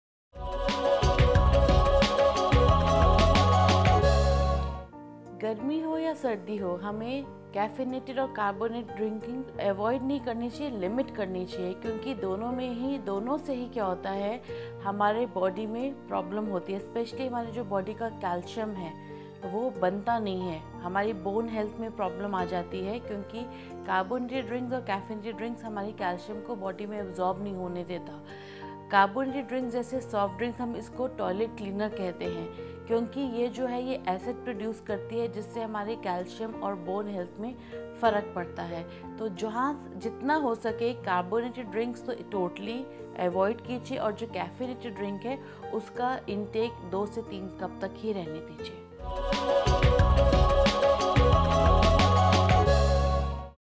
Harmful Effects Of Soft Drinks | Avoid Aerated Drinks | Dr. Rashmi Bhatia (Dietitian)
Dr. Rashmi Bhatia (Dietitian) Tells Us Harmful Effects Of Soft Drinks | Avoid Aerated Drinks |